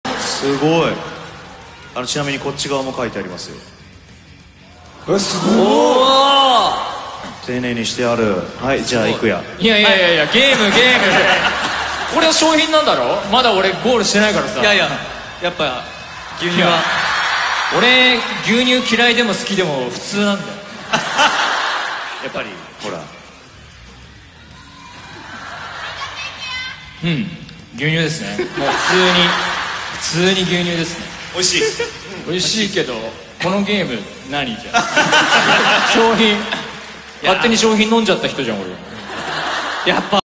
[ENG SUB] Uchiyama Kouki - the Iwatobi Milk Taste Tester
Video Clip frok High Speed Event

Iwatobi Game Corner:
At this clip Zakki and Tattsun had an Iwatobi Milk as their prize and Tattsun showed everyone the nutrition facts written on the carton.